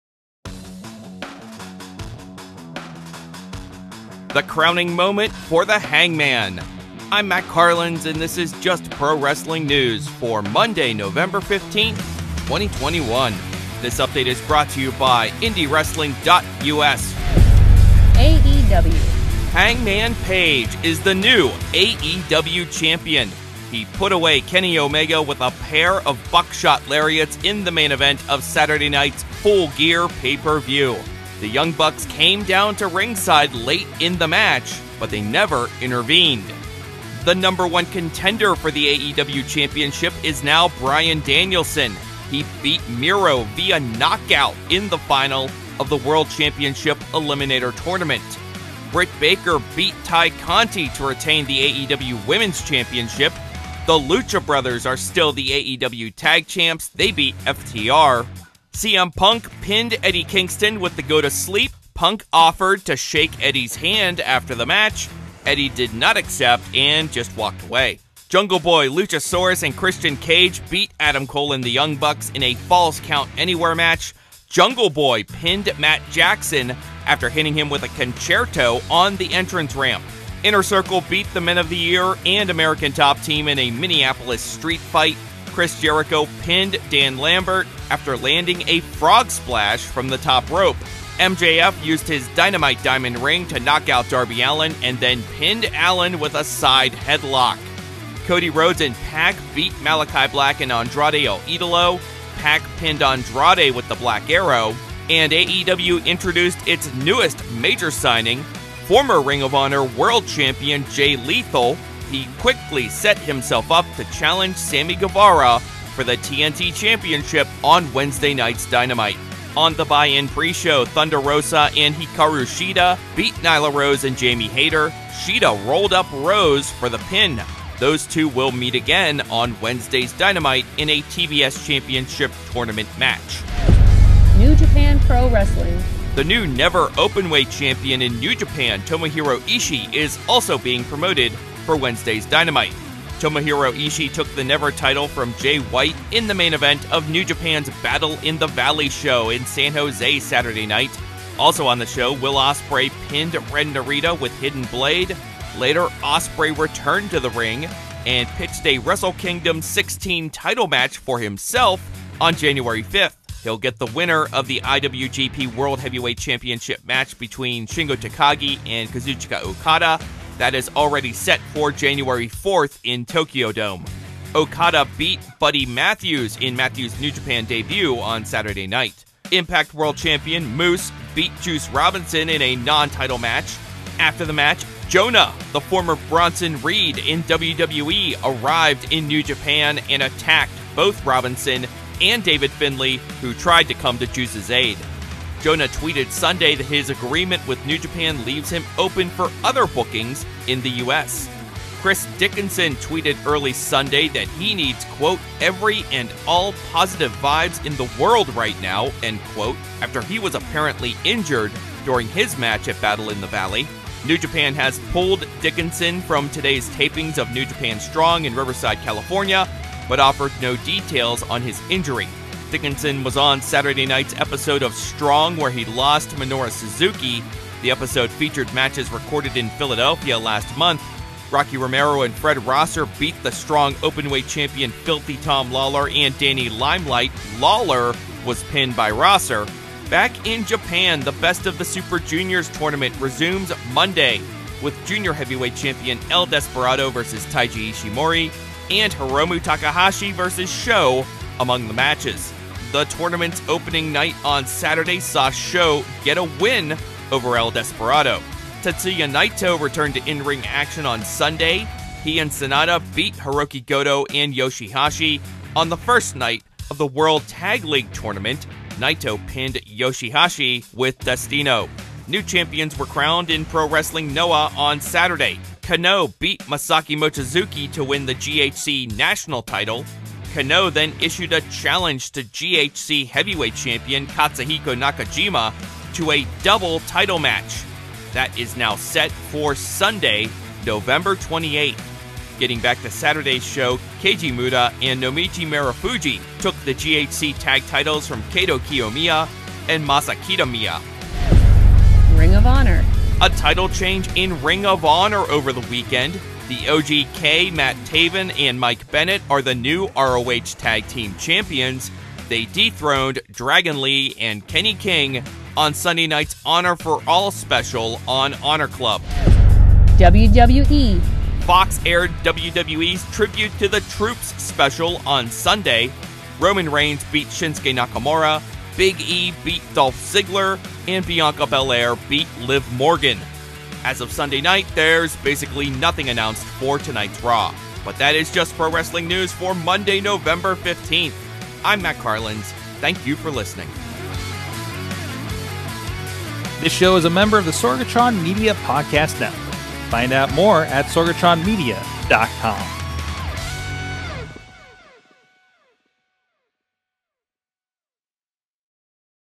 AEW Full Gear Results. Major New AEW Signing.
I’m Matt Carlins and this is JUST Pro Wrestling News for Monday, November 15, 2021.

This update is brought to you by IndyWrestling.us, where you can see the latest from the Renegade Wrestling Alliance - Open Season - featuring Ring Of Honor’s O’Shay Edwards and Impact Wrestling’s Crazzy Steve and Sam Beale.

(**STINGER: AEW**)

Hangman Page is the new AEW Champion. He put away Kenny Omega with a pair of Buckshot Lariats in the main event of Saturday night’s Full Gear pay per view. The Young Bucks came down to ringside late in the match but never intervened.

The #1 contender for the AEW Championship is Bryan Danielson. He beat Miro via knockout in the final of the World Championship Eliminator Tournament.

Dr. Britt Baker beat Tay Conti to retain the AEW Women’s Championship.

The Lucha Brothers are still the AEW Tag Champs. They beat FTR.

CM Punk pinned Eddie Kingston with the Go 2 Sleep. Punk offered to shake Eddie’s hand after the match. Eddie did not accept and just walked away.

Jungle Boy, Luchasaurus & Christian Cage beat Adam Cole & the Young Bucks in a Falls Count Anywhere Match. Jungle Boy pinned Matt Jackson after hitting him with a Conchairto on the entrance ramp.

Inner Circle beat The Men Of The Year & American Top Team in a Minneapolis Street Fight. Chris Jericho pinned Dan Lambert after landing a frog splash from the top rope.

MJF used his Dynamite Diamond Ring to knock out Darby Allin, and then pinned Allin with a side headlock.

Cody Rhodes & Pac beat Malakai Black & Andrade El Idolo. Pac pinned Andrade with the Black Arrow.

AEW introduced its newest major signing - former Ring Of Honor World Champion Jay Lethal. He quickly set himself up to challenge Sammy Guevara for the TNT Championship on Wednesday night’s Dynamite.

On the Buy In preshow…Thunder Rosa & Hikaru Shida beat Nyla Rose & Jamie Hayter. Shida rolled up Rose for the pin. Those two will meet again on Wednesday’s Dynamite in a TBS Championship Tournament match.

The NEW NEVER Openweight Champion in New Japan - Tomohiro Ishii - is also being promoted for Wednesday’s Dynamite.

(**STINGER: New Japan**)

Tomohiro Ishii took the NEVER Title from Jay White in the main event of Saturday night’s Battle In The Valley show in San Jose.

Also on the show, Will Ospreay pinned Ren Narita with Hidden Blade.

Later, Ospreay returned to the ring...and pitched a Wrestle Kingdom 16 title match for himself on January 5th. He’ll get the winner of the IWGP World Heavyweight Championship Match between Shingo Takagi and Kazuchika Okada that’s already set for January 4th in Tokyo Dome..

Okada beat Buddy Matthews in Matthews’ New Japan debut Saturday night.

Impact World Champion Moose beat Juice Robinson in a non-title match.

After the match, Jonah - the former Bronson Reed in WWE - arrived in New Japan and attacked both Robinson and David Finlay, who tried to come to Juice’s aid.

Jonah tweeted Sunday that his agreement with New Japan leaves him open for other bookings in the U.S.

Chris Dickinson tweeted early Sunday that he needs “every and all positive vibes in the world right now” after he was apparently injured during his match at Battle In The Valley.

New Japan pulled Dickinson from today’s tapings of New Japan Strong in Riverside, California...but offered no details on his injury.

Dickinson was on Saturday night’s episode of Strong, where he lost to Minoru Suzuki.

The episode featured matches recorded in Philadelphia last month.

Rocky Romero & Fred Rosser beat the Strong Openweight Champion FILTHY Tom Lawlor & Danny Limelight. Lawlor was pinned by Rosser.

Back in Japan…

The Best Of The Super Juniors tournament resumes Monday...with Jr. Heavyweight Champion El Desperado vs. Taiji Ishimori...and Hiromu Takahashi vs. SHO among the matches.

The tournament’s opening night on Saturday saw SHO get a win over El Desperado.

Tetsuya Naito returned to in-ring action on Sunday. He and SANADA beat Hirooki Goto & YOSHI-HASHI on the first night of the World Tag League tournament. Naito pinned YOSHI-HASHI with Destino.

New champions were crowned at Pro-Wrestling NOAH’s show on Saturday.

Kenoh beat Masaaki Mochizuki to win the GHC National Title. Kenoh then challenged GHC Heavyweight Champ Katsuhiko Nakajima to a Double Title Match. That is now set for Sunday, November 28th.

Also on Saturday, Keiji Muto & Naomichi Marufuji took the GHC Tag Titles from Kaito Kiyomiya & Masa Kitamiya. Muto pinned Kitamiya.